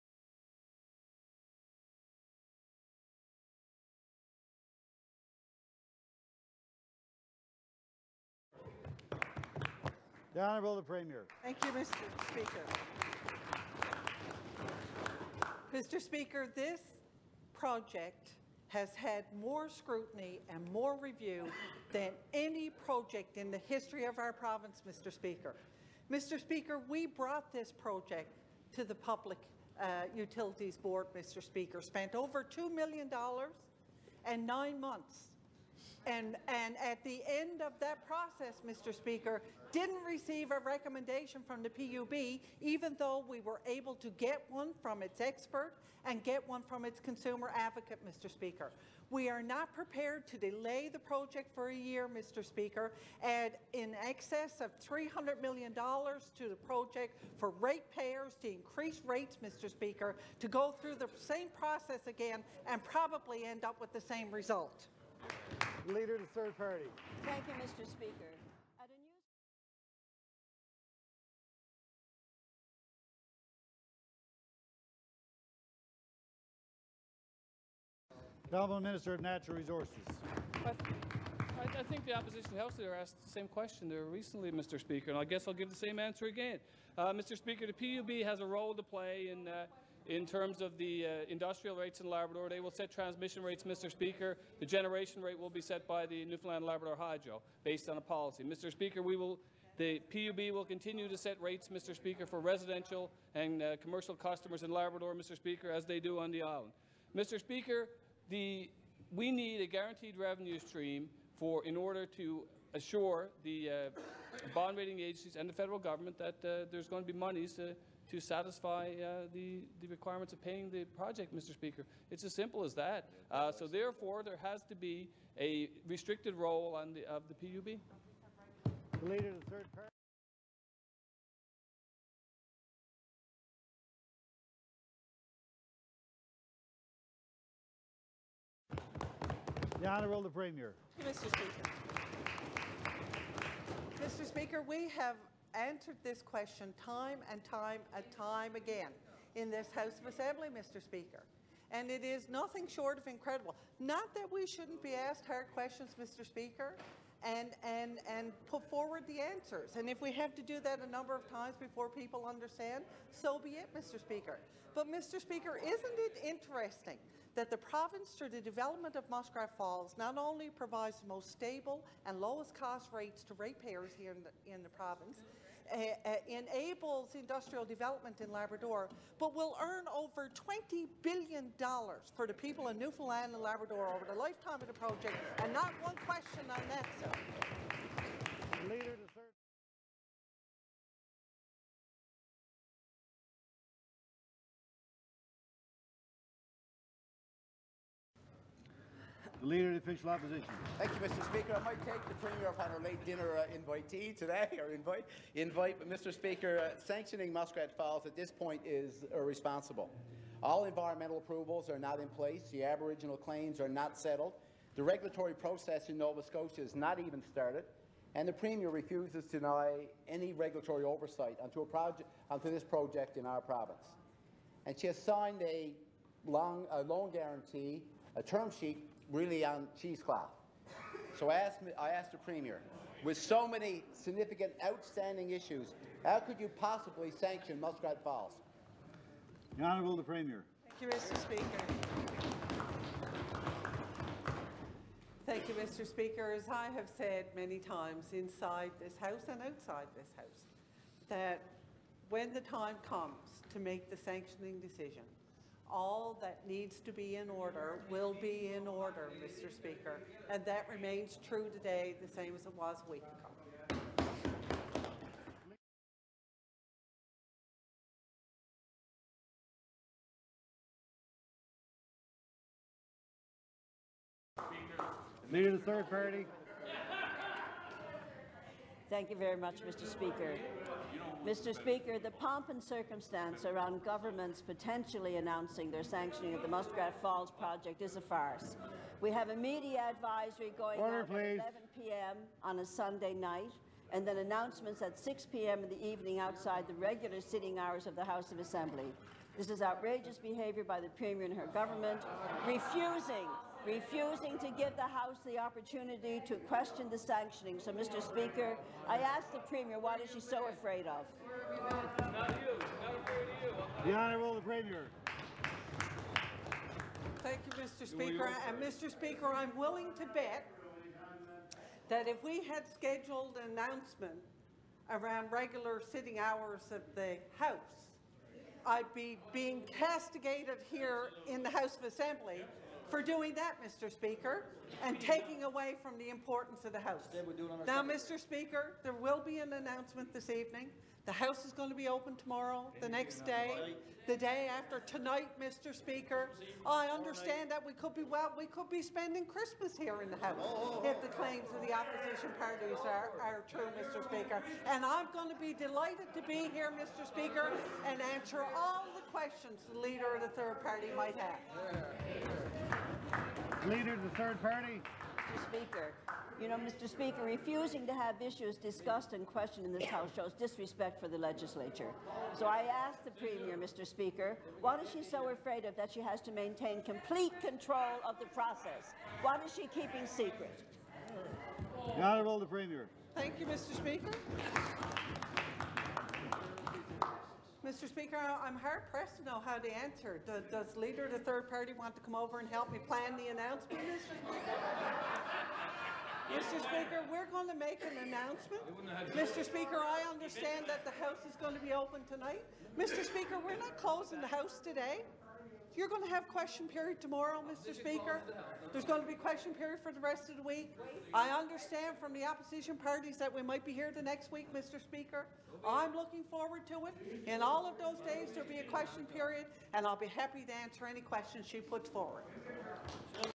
Muskrat Falls sanctioning, NL House of Assembly clips
A selection of clips from November/December, 2012, around the sanctioning of the Muskrat Falls project in the Newfoundland and Labrador House of Assembly. Statements from Premier Kathy Dunderdale, Minister of Natural Resources Jerome Kennedy, Leader of the Official Opposition Dwight Ball, and Leader of the 3rd Party Lorraine Michael. These are clips for a documentary project, Muskrat Falls: Power and Resistance.